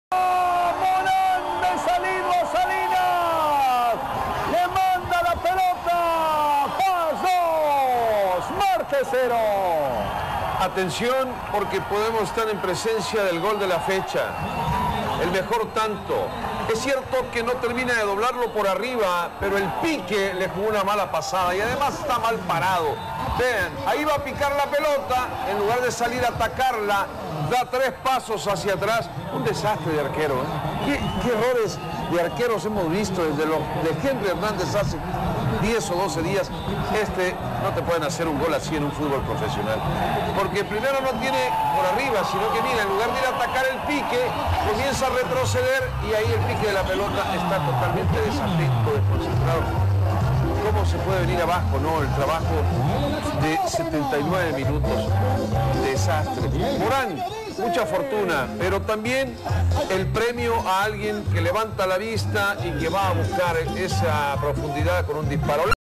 Gol de Jorge Morán / C.D. FAS 2-0 C.D. Atlético Marte / Fútbol Salvadoreño
¡Goooool de FAS! Gol de Jorge Morán
Liga Mayor de Fútbol Salvadoreño
Jornada 15 del Torneo Clausura 2015 de la Liga Pepsi
Estadio Oscar Quiteño
18/Abril/2015